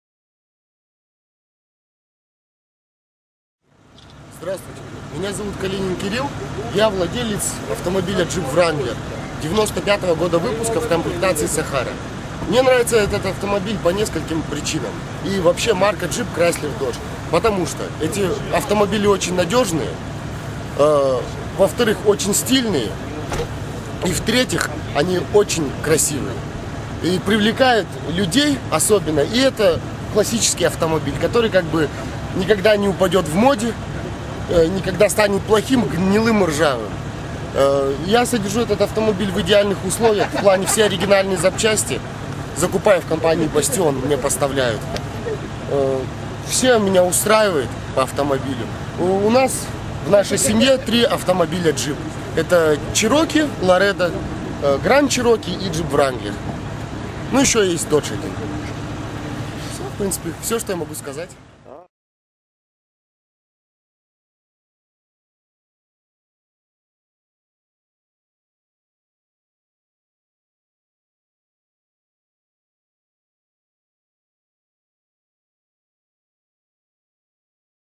"Мой джип" Jeep Club Kazakhstan Astana
New Jeep Club started in Kazakhstan with Astana team getting together on the steppe for some play time in the mud.  Everyone had fun!  Hear one owner talk about why he loves his Jeep.